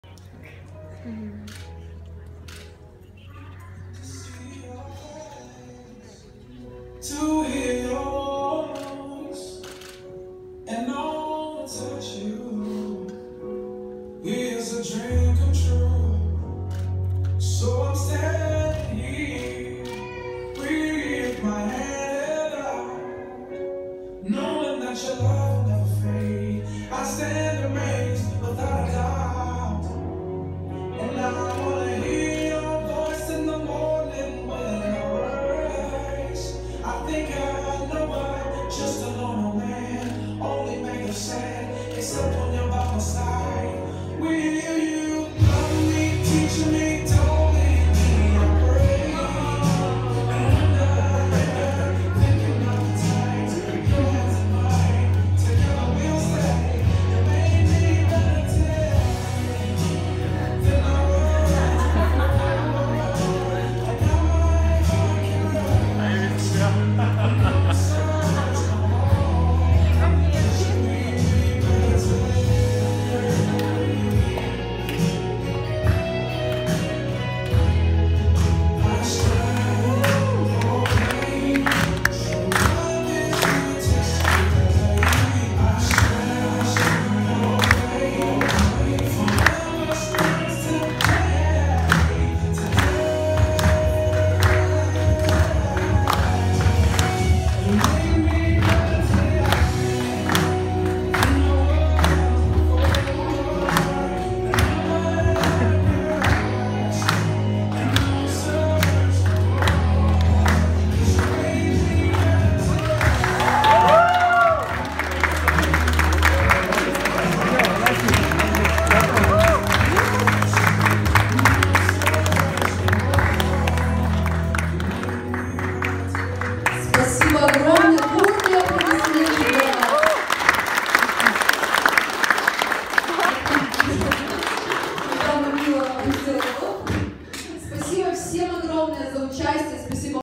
Трогательный свадебный танец
10 января 2016 прекрасная свадьба Ангелины и Евгения